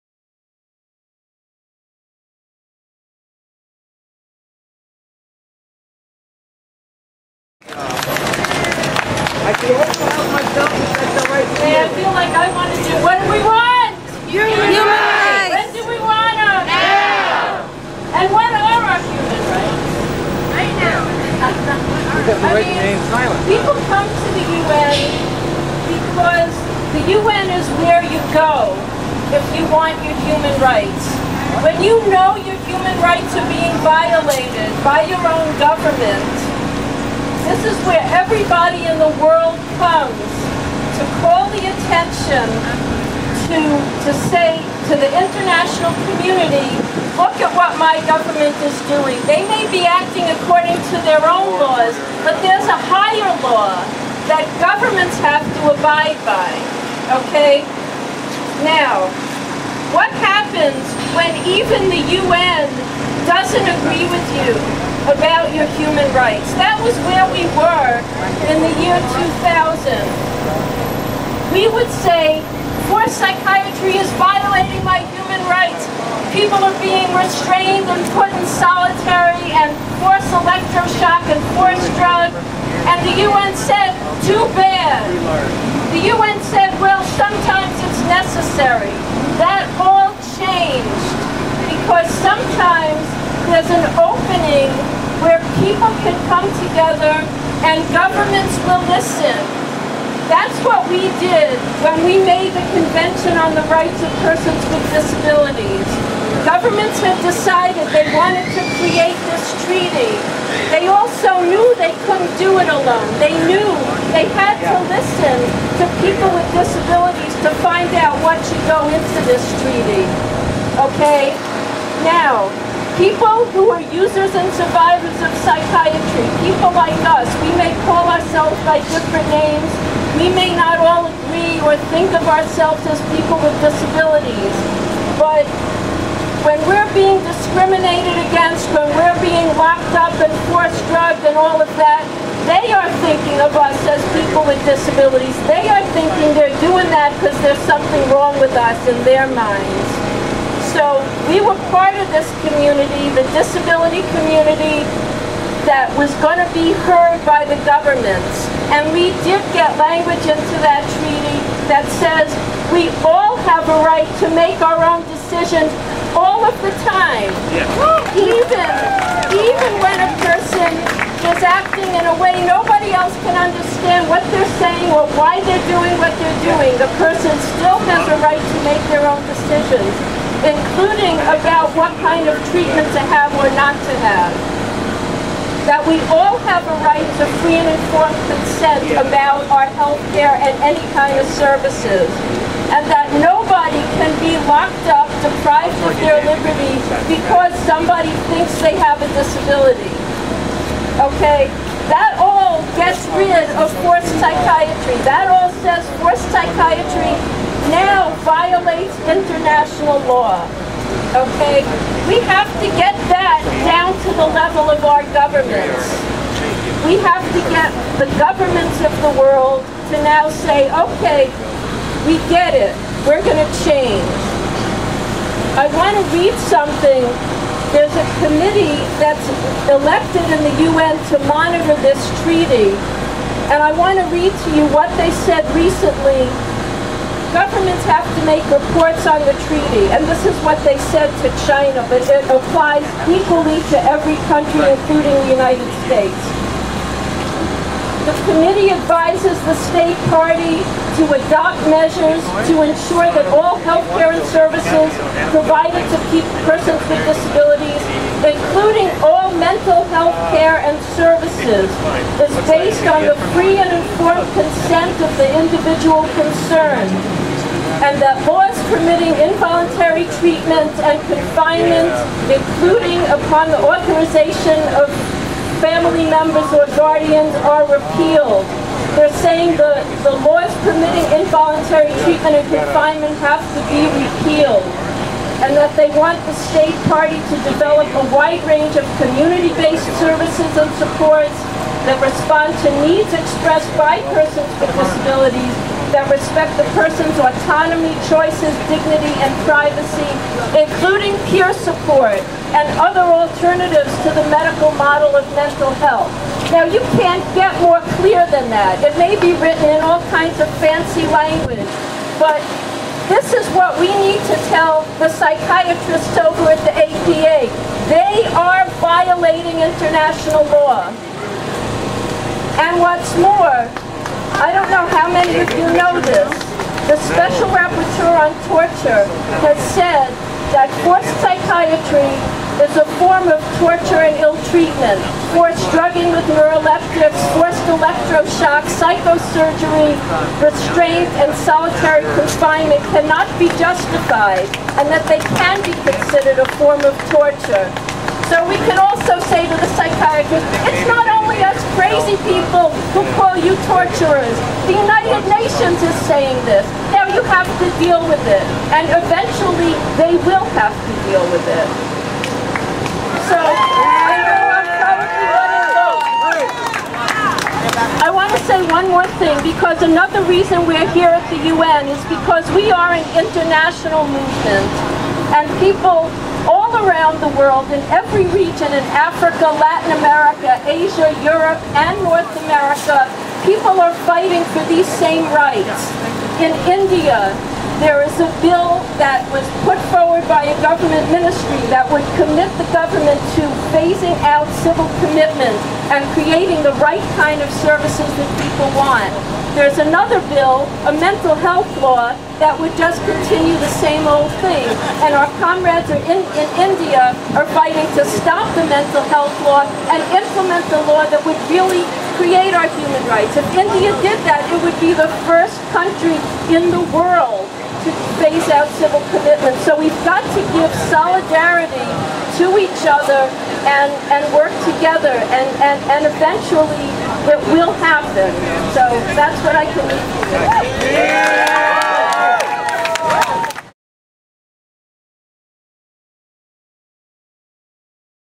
Tina Minkowitz, JD - Occupy the APA - October 2012
Tina Minkowitz, JD, Director of the Center for the Human Rights of Users and Survivors of Psychiatry, gives a speech on human rights in "mental health" at the Occupy the APA Rally and March from the UN to the Annual Meeting of the American Psychiatric Association.